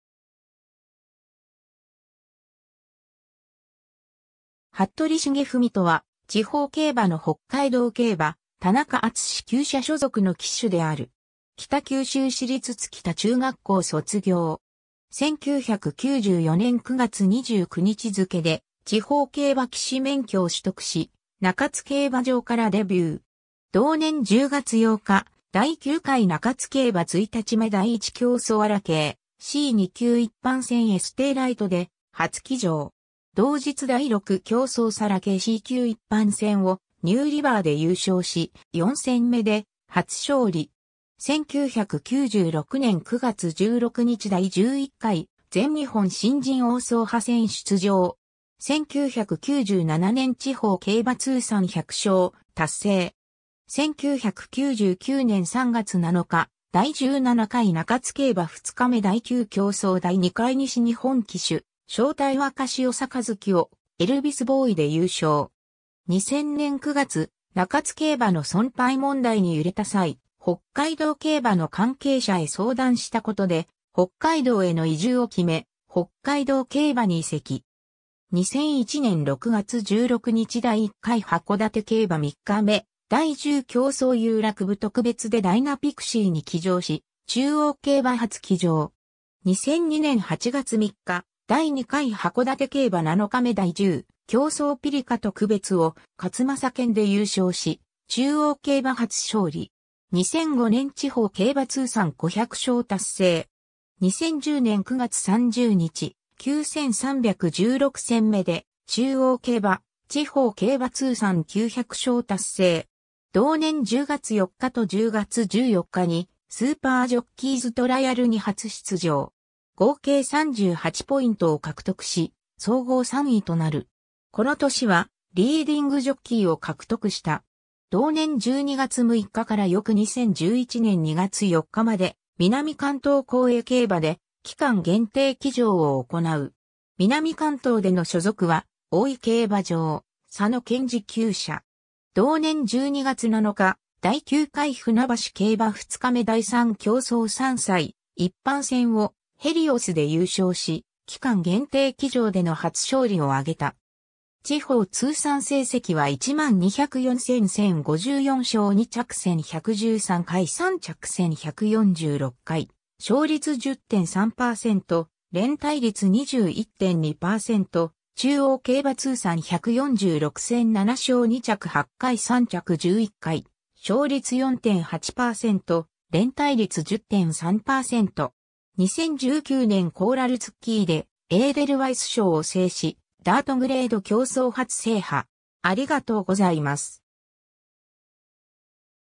服部茂史
服部 茂史（はっとり たかふみ、1976年3月14日 - ）とは、地方競馬のホッカイドウ競馬・田中淳司厩舎所属の騎手である。
北九州市立槻田中学校卒業。
 1994年9月29日付けで地方競馬騎手免許を取得し、中津競馬場からデビュー。
同年10月8日第9回中津競馬1日目第1競走アラ系C2級一般戦エステーライトで初騎乗（8頭立て1番人気3着）。
同日第6競走サラ系C級一般戦をニューリヴァーで優勝（8頭立て1番人気）し、4戦目で初勝利。
 1996年9月16日第11回全日本新人王争覇戦出場（12人中4位）。
 1997年地方競馬通算100勝達成。
 1999年3月7日第17回中津競馬2日目第9競走第2回西日本騎手招待若潮杯（C級）をエルビスボーイで優勝（9頭立て3番人気）。
 2000年9月、中津競馬の存廃問題に揺れた際、ホッカイドウ競馬の関係者へ相談した事で北海道への移住を決めホッカイドウ競馬に移籍。
 2001年6月16日第1回函館競馬3日目第10競走遊楽部特別（3歳以上牝馬限定500万下）でダイナピクシーに騎乗（16頭立て16番人気16着）し、中央競馬初騎乗。
 2002年8月3日第2回函館競馬7日目第10競走美利河特別（3歳以上500万下）をカツマサケンで優勝（16頭立て16番人気）し、中央競馬初勝利。
 2005年地方競馬通算500勝達成。
 2010年9月30日、9316戦目で中央競馬・地方競馬通算900勝達成。
同年10月4日と10月14日にスーパージョッキーズトライアルに初出場。
合計38ポイントを獲得し総合3位となる。
 この年はリーディングジョッキーを獲得した。
 同年12月6日から翌2011年2月4日まで南関東公営競馬で期間限定騎乗を行う。
南関東での所属は大井競馬場・佐野謙二厩舎。
同年12月7日第9回船橋競馬2日目第3競走3歳一般戦（80.1万円以上125.5万円未満）をヘリオスで優勝（14頭立て1番人気）し、期間限定騎乗での初勝利をあげた。
 地方通算成績は10204戦1054勝・2着1113回・3着1146回・勝率10.3%・連対率21.2%（2011年12月31日現在）、中央競馬通算146戦7勝・2着8回・3着11回・勝率4.8%・連対率10.3%。
 2019年コーラルツッキーでエーデルワイス賞を制し、ダートグレード競走初制覇。